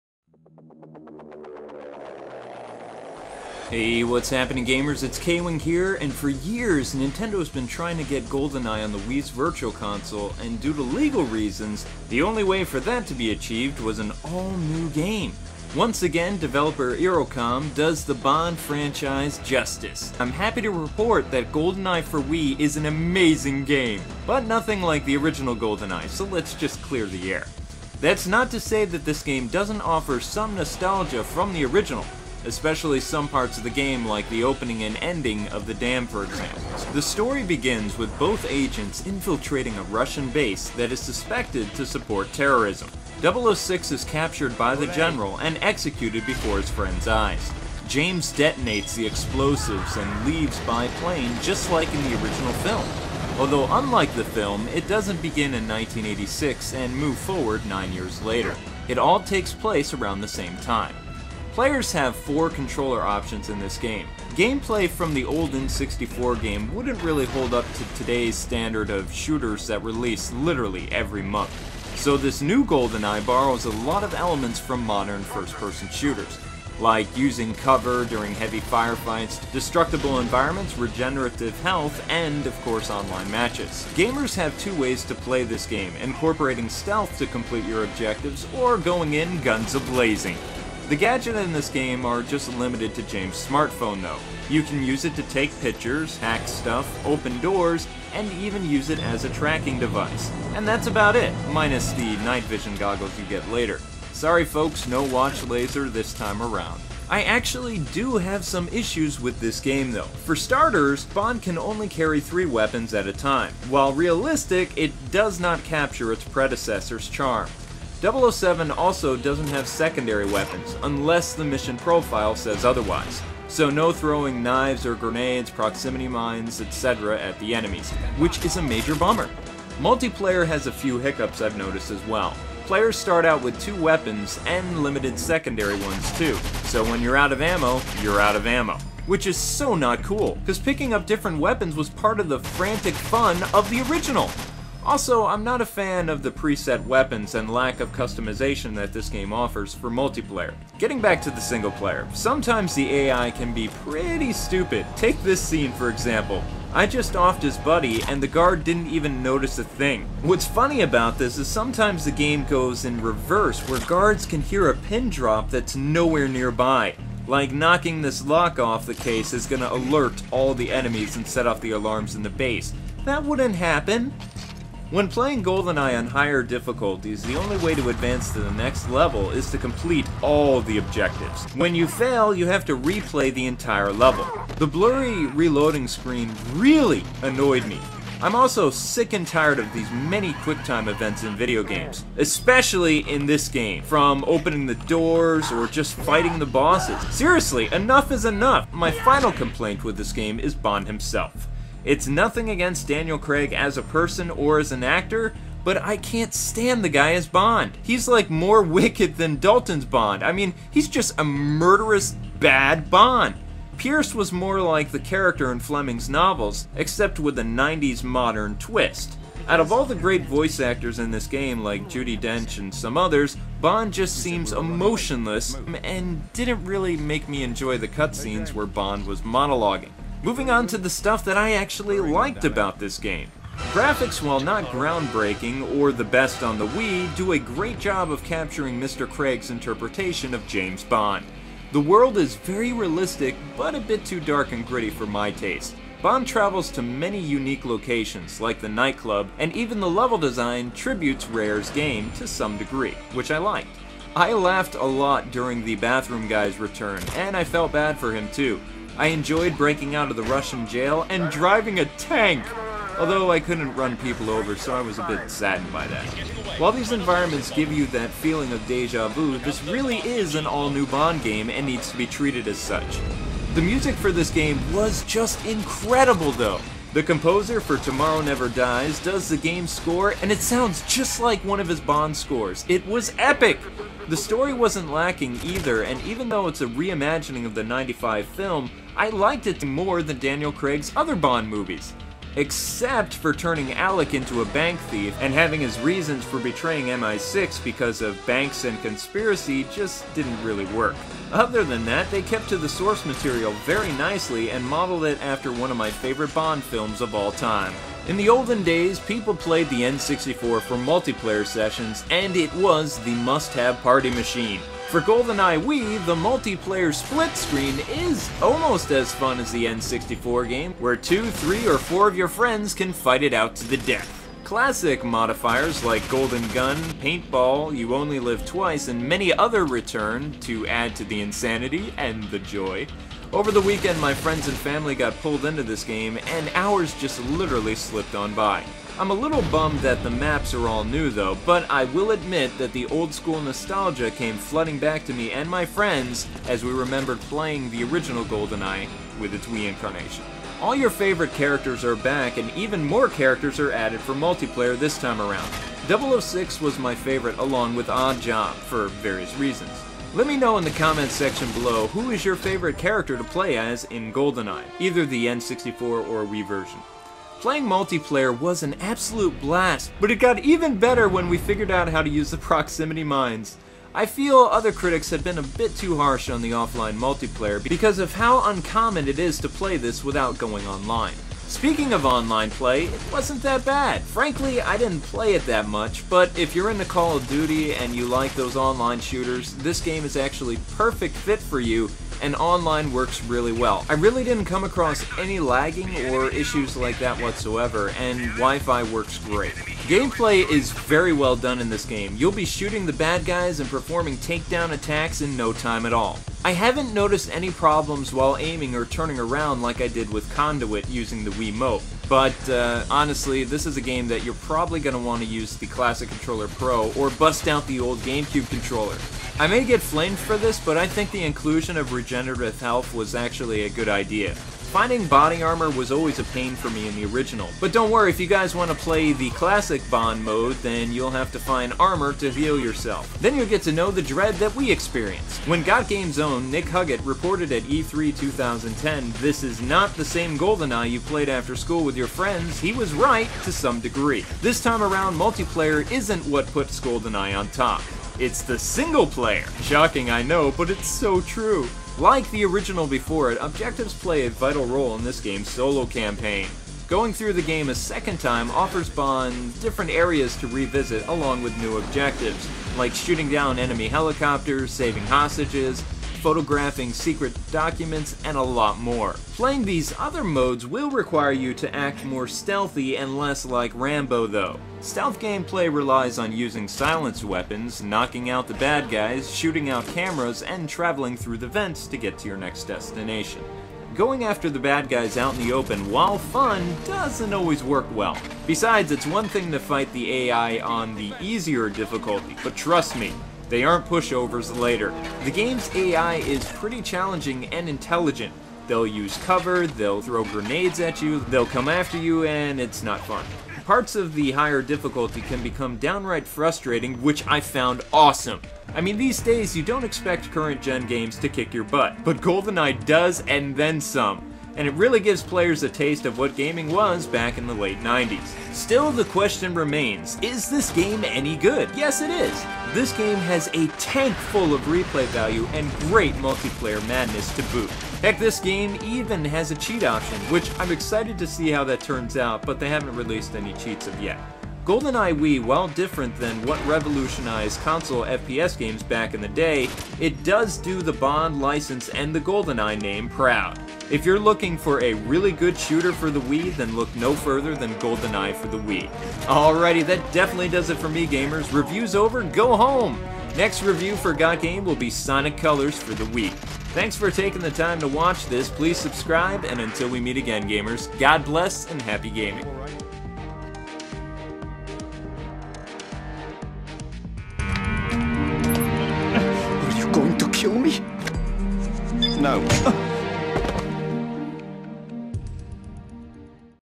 GoldenEye 007 Review (Wii)
GoldenEye 007 Wii Video Review! Can Bond strike Gold again? Check out GotGame's review to find out.